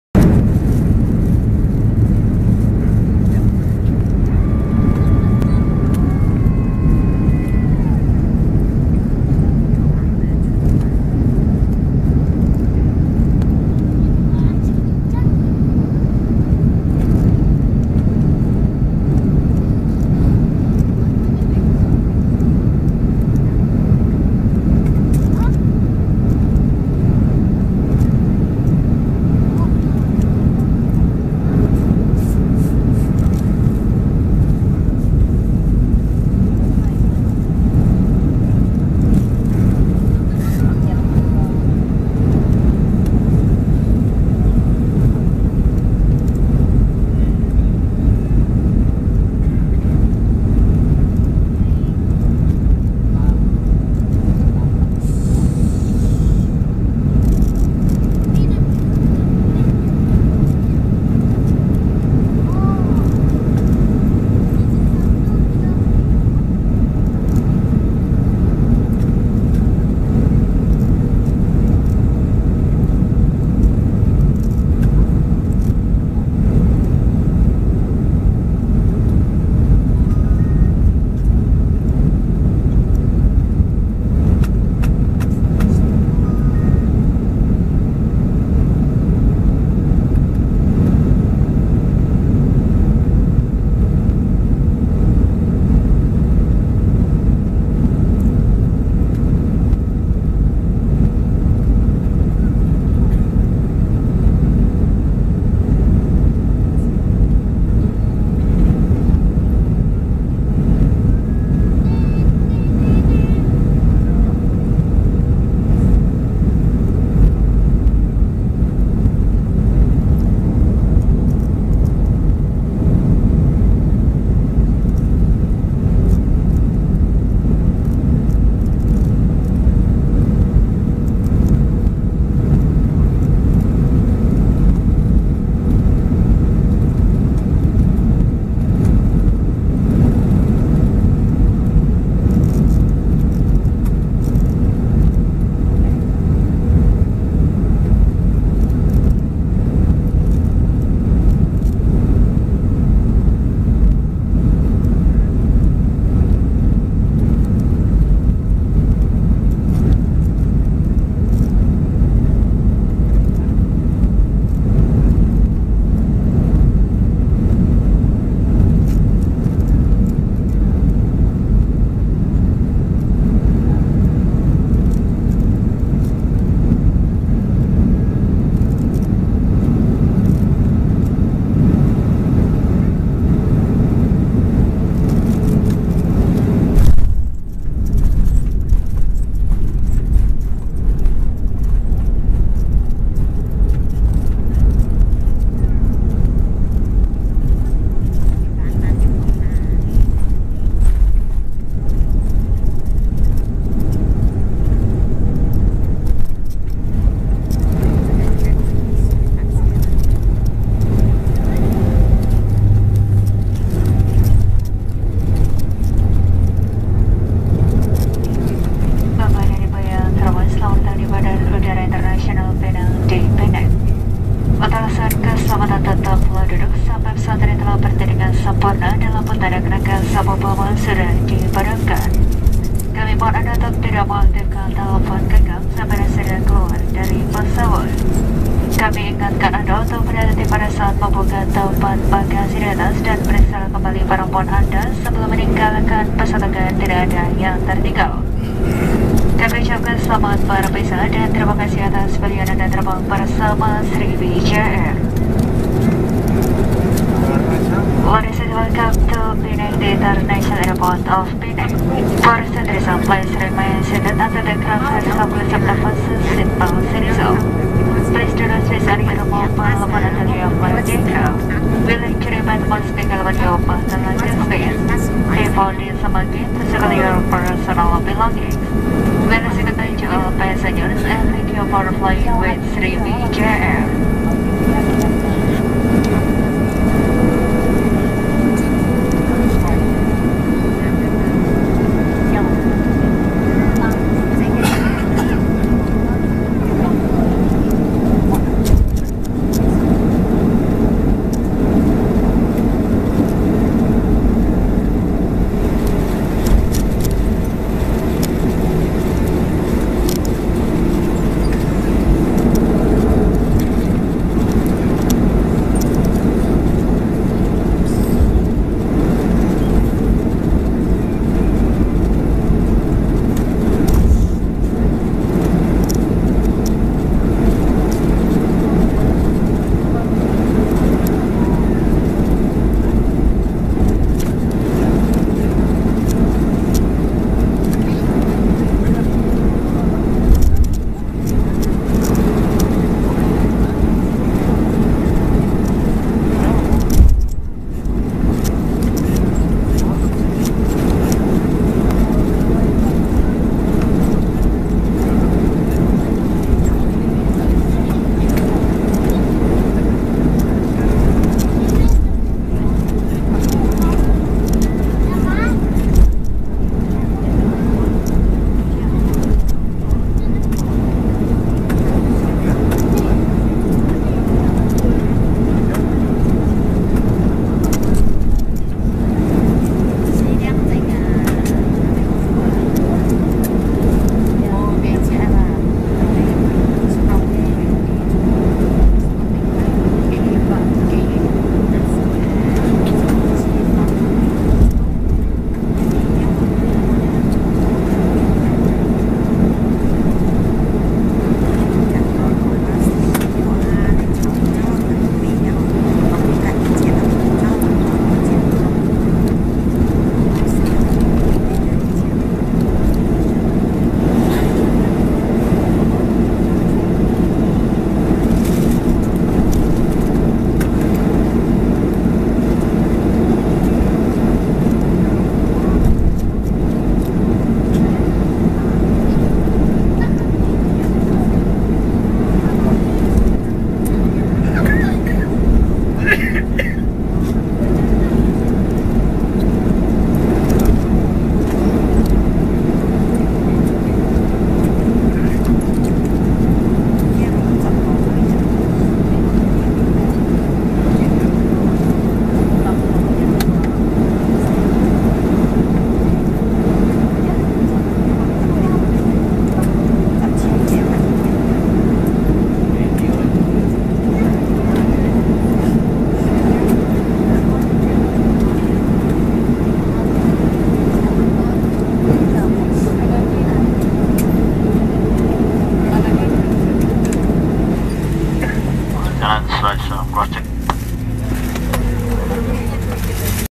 (RARE) Sriwijaya Air Boeing 737-400 Landing at Penang
This is the landing part of the 47 minutes hop from Medan to Penang.
For the best resolution, please watch it in HD (720p or 1080p).

DETAILS ABOUT THIS FLIGHT

AIRLINE: Sriwijaya Air
AIRCRAFT: Boeing 737-400
REGISTRATION: PK-CKC (Nosename: Agung)
FLIGHT: 102
ORIGIN: Medan (Kualanamu International Airport - KNO)
VIA: -
DESTINATION: Penang (Penang International Airport / Bayan Lepas International Airport - PEN)
DATE: 02-January-2013
DEPART: 08:55LT (SCHEDULED) - 09:42LT (ACTUAL)
ARRIVE: 10:55LT (SCHEDULED) - 11:29LT (ACTUAL)
FLIGHT TIME: 1 Hour (SCHEDULED) - 47 Minutes (ACTUAL)
ALTITUDE: 19000FT
CAPTAIN IN COMMAND: Unknown